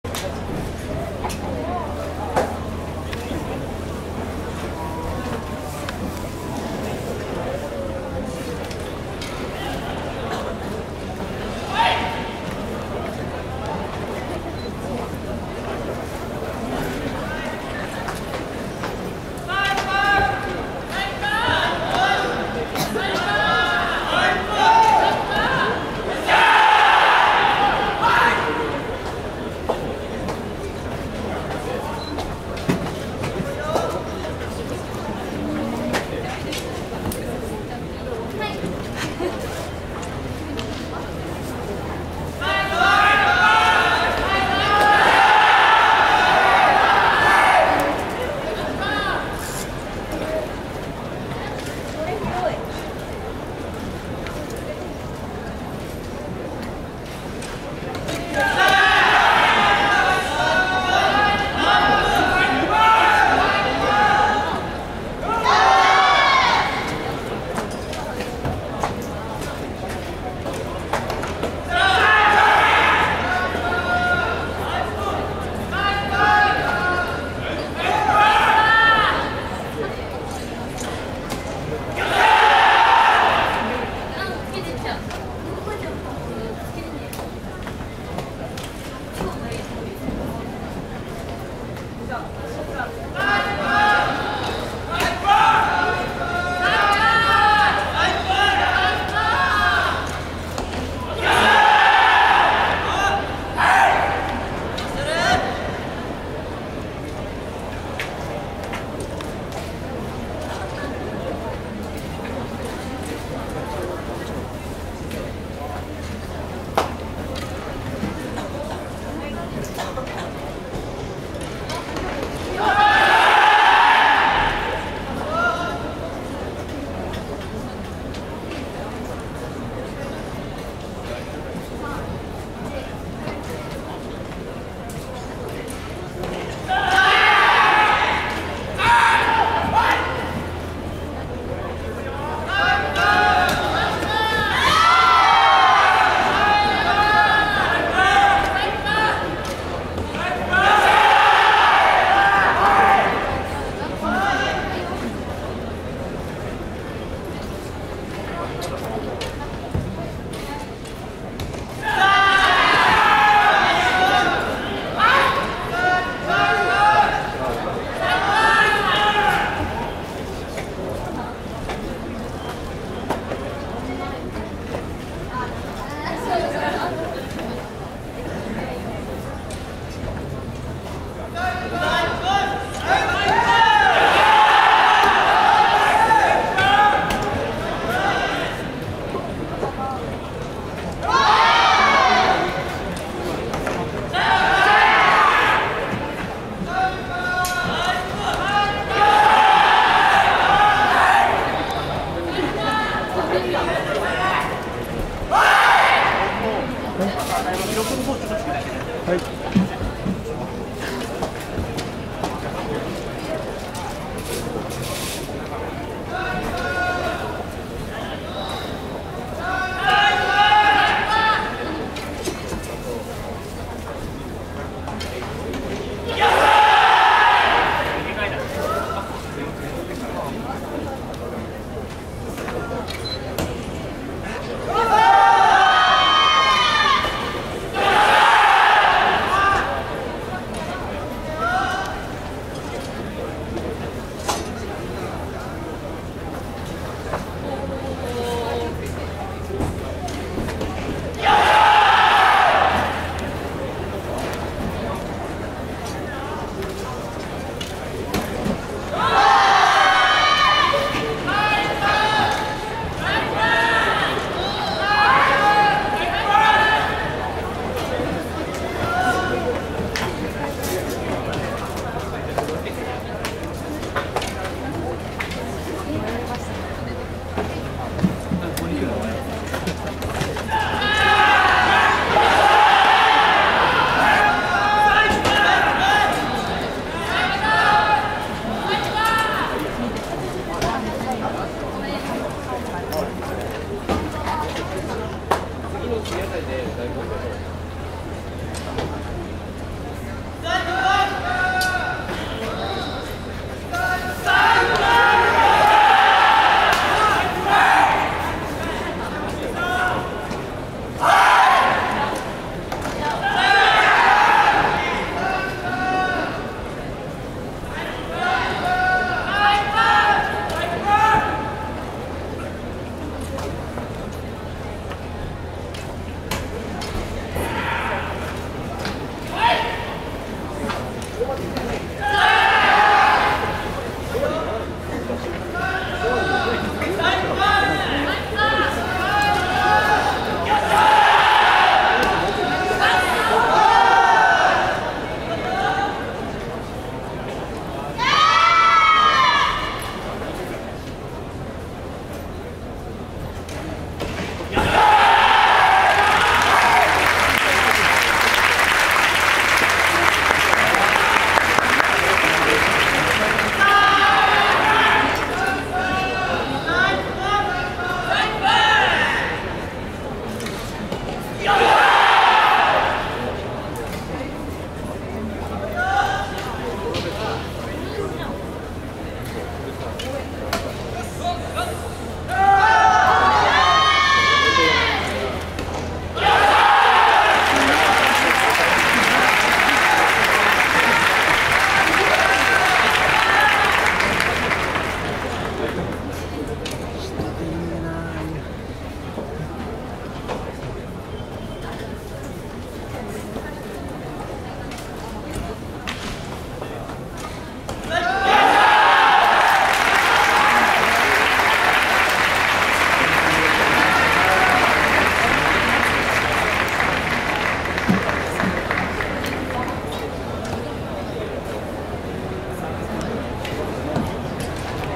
2014 All Kanto College Championship Tournament Nihon University (Men)
44th All Kanto Student Kyudo Championships
June 20.21, 2014.
Venue: Nippon Budokan
Nihon University (Men)

All Kanto College Championship Tournament
Participated by Universities from Kanto region
Preliminary competition to the following All Japan Competition
Tremendous honor to winner college

⦁ Tournoi d’étudiants de la région Kanto de Kyudo
C’est le tournoi dans lequel les universités de la région Kanto se rencontrent. Le tournoi est considéré comme escarmouche avant le championnat national et il donne grand honneur à l’équipe gagnante.

kyudo Japanese archery budo